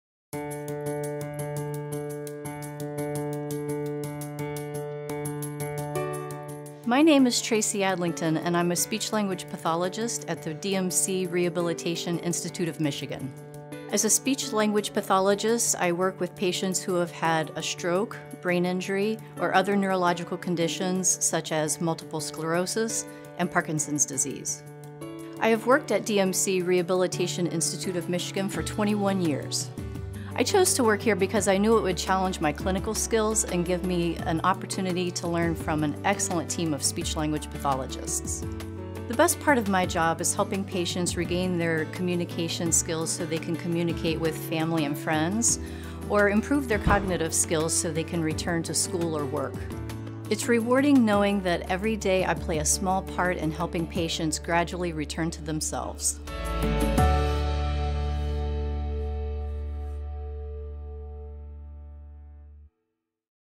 Behind the Mask: Speech-Language Pathologist | DMC Rehabilitation Institute of Michigan
Tracy Adlington has been providing compassionate care as a speech-language pathologist at DMC Rehabilitation Institute of Michigan for over 21 years now. In this video, she shared what she loves most about her work and why she has chosen to work at DMC. We commend Tracy for her passion for service and commitment to providing high-quality care for the communities we serve.

And as always, your safe care is our #1 priority.